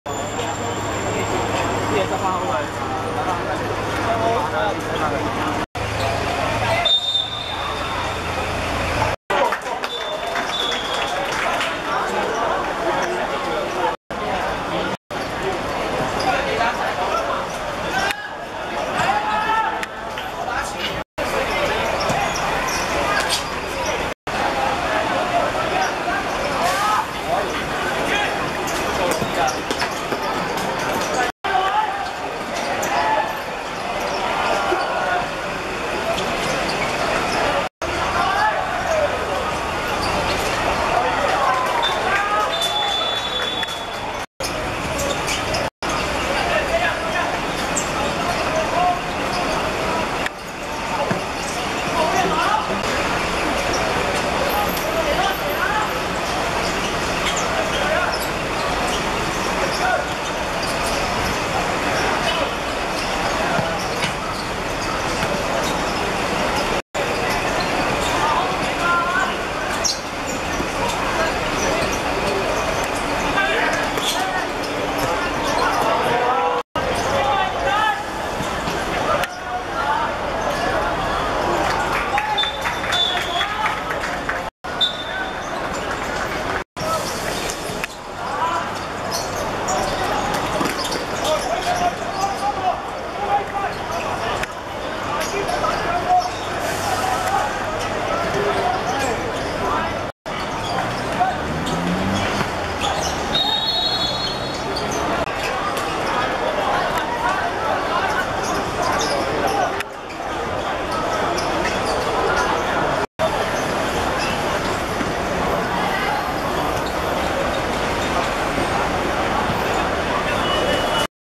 車路士A vs 愉園(2014 NIKE 5 U16八強)精華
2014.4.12.麥花臣球塲 15:30 車路士A(綠) 3:0 愉園(藍)